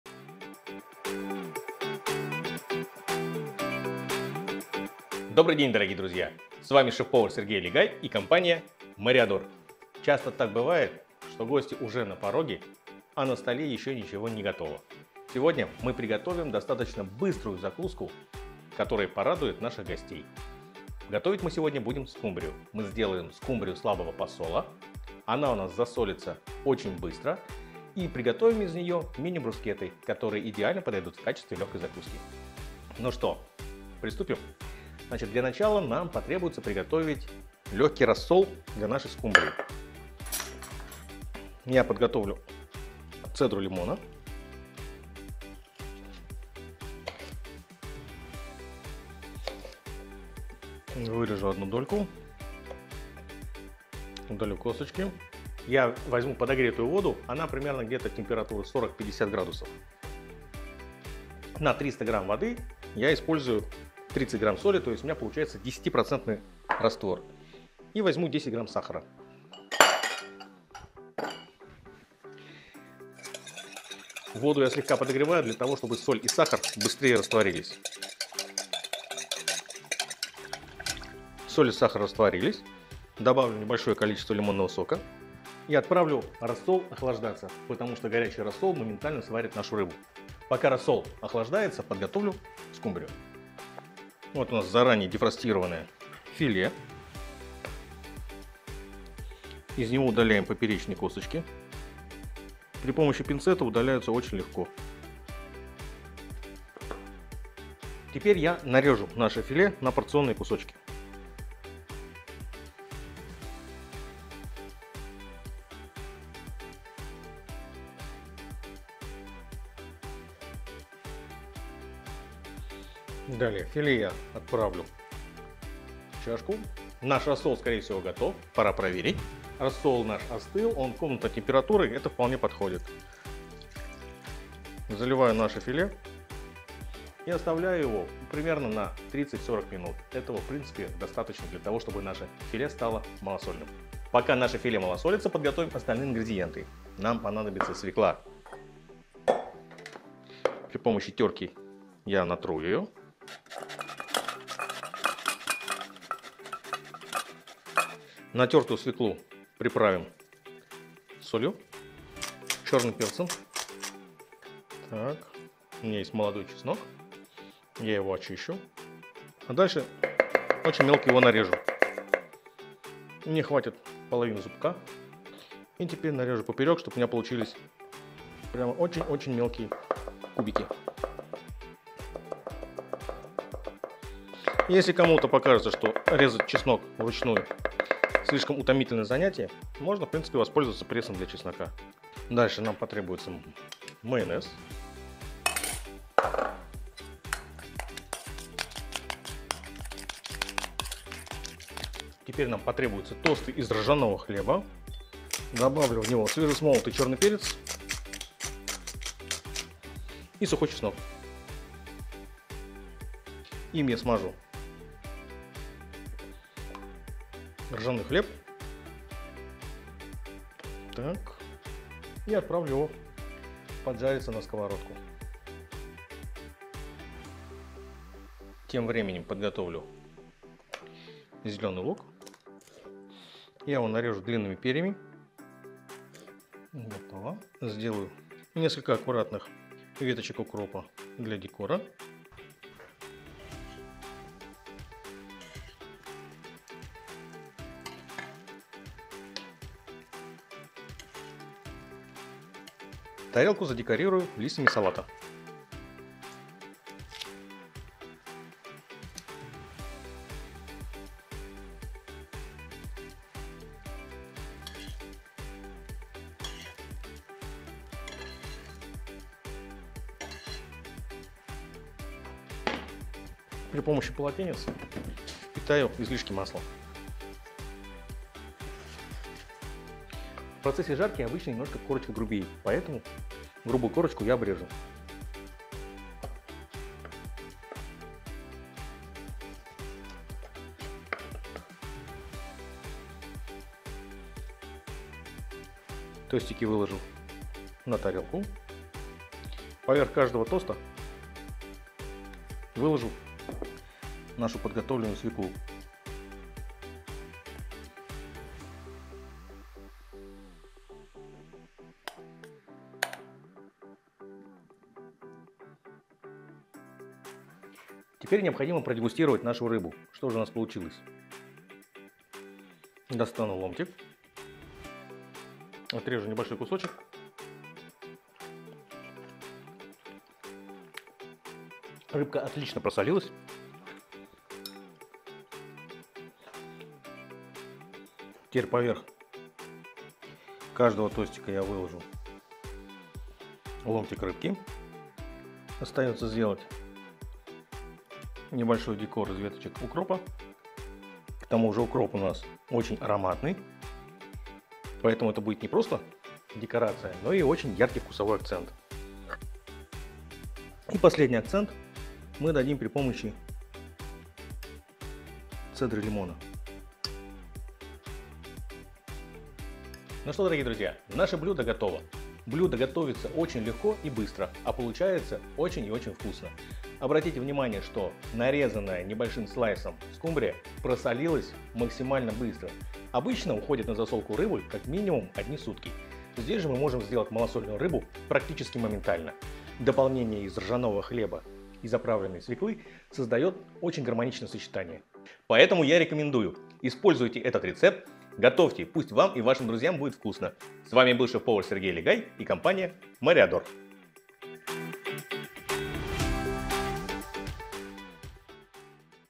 Брускетта со скумбрией. Быстрая и вкусная закуска: как приготовить бутерброд с рыбой и хлебом
Добрый день, дорогие друзья! С вами шеф-повар Сергей Лигай и компания МОРЕОДОР. Сегодня у нас для вас отличный рецепт, который поможет вам подготовить вкусную закуску всего за несколько минут. Если вы не хотите тратить много времени на сложные блюда, но хотите удивить своих гостей, этот рецепт идеально подходит для вас. Мы готовим малосольную скумбрию, которая засаливается буквально за 30 минут! Это быстрый способ приготовить рыбу, не ожидая сутки, как обычно требуется для традиционного посола.

Для начала мы приготовим легкий рассол для нашей скумбрии. Вода, подогретая до температуры 40-50 градусов, соль, сахар и лимонный сок — эти простые ингредиенты помогут сделать рыбу малосольной буквально за полчаса. Этот способ значительно ускоряет процесс засолки и позволяет нам получить потрясающий вкус без длительного ожидания.

Затем мы подготовим филе скумбрии, удалим из него все кости при помощи пинцета и нарежем на порционные кусочки. После того как рыба будет готова, заливаем ее подготовленным рассолом и оставляем на 30-40 минут. Это время достаточно, чтобы рыба пропиталась вкусом рассола и стала идеальной для подачи.

Пока наша скумбрия засаливается, займемся другими компонентами блюда. Мы будем использовать свежую свеклу, которую натрем на терке и приправим солью и черным перцем. Свекла придаст яркость и необычный вкус нашему блюду. Добавим немного чеснока, нарежем его мелкими кубиками, и вместе с майонезом создадим отличную заправку для свеклы. Далее нарежем ржаной хлеб на тосты, добавим к нему черный перец и сухой чеснок, чтобы придать дополнительный аромат.

Теперь, когда все готово, мы обжариваем тосты на сковороде и выкладываем на них свеклу, а сверху кладем ломтики засоленной скумбрии. Для финального акцента мы добавим веточки укропа, цедры лимона и немного оливкового масла, чтобы усилить аромат блюда.

Это легкая, но невероятно вкусная закуска идеально подойдет для любого мероприятия. Малосольная скумбрия, поданная с ржаным хлебом, свеклой и укропом, — это идеальное сочетание для тех, кто ценит быстрые и оригинальные рецепты.

Простой и быстрый способ приготовить рыбу, который не только сэкономит вам время, но и станет настоящим хитом на вашем столе. Так что, не теряйте времени — попробуйте этот рецепт и поделитесь им с друзьями!

Не забывайте подписываться на канал МОРЕОДОР, чтобы не пропустить новые рецепты от Сергея Лигая и другие кулинарные советы. Увидимся в следующем видео!